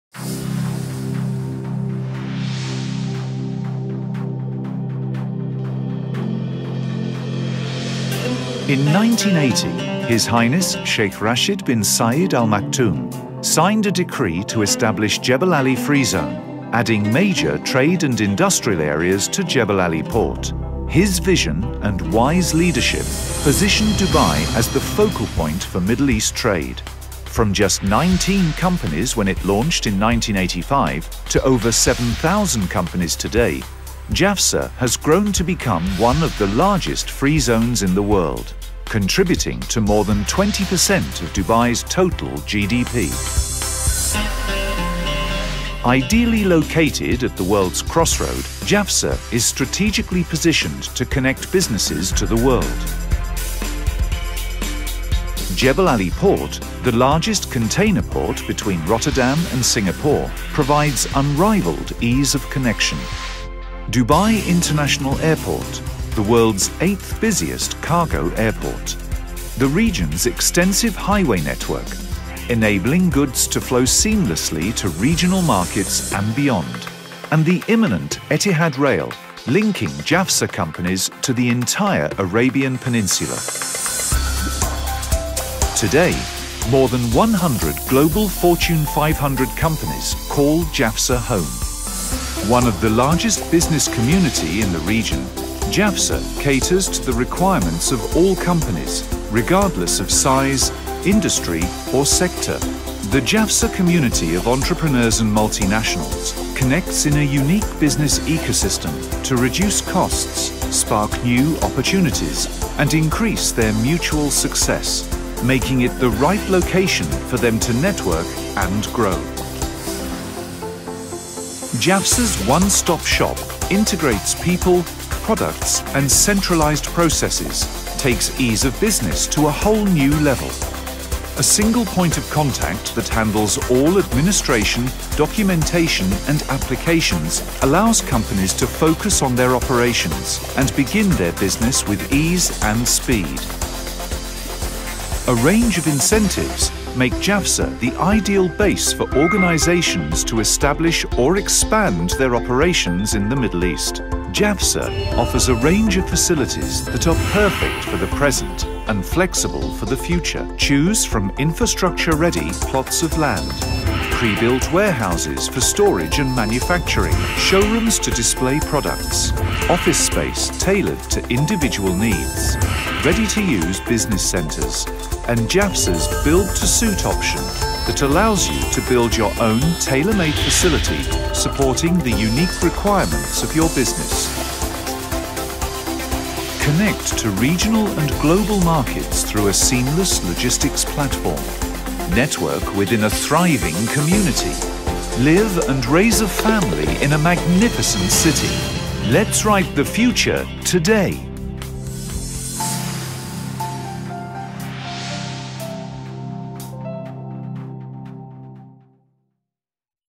Why Dubai's Jebel Ali Freezone(JAFZA)
Jebel Ali Free Zone (JAFZA) which was founded in  1985, as part of the vision of Dubai ruler  H.H Sheikh Rashid Bin Saeed , have matured into an ever expanding business platform that is ready for today.
JAFZA have grown from a small operation of just 19 companies into a thriving business community with over 7,000 companies from over 100 countries ,that continues to expand.  Today, Jafza sustaining over 144,000 jobs and attracting more than 32% of the UAE’s foreign direct investment; all the while exceeding 50% of Dubai’s total exports, with an phenomenal value for trade of $87.6 billion.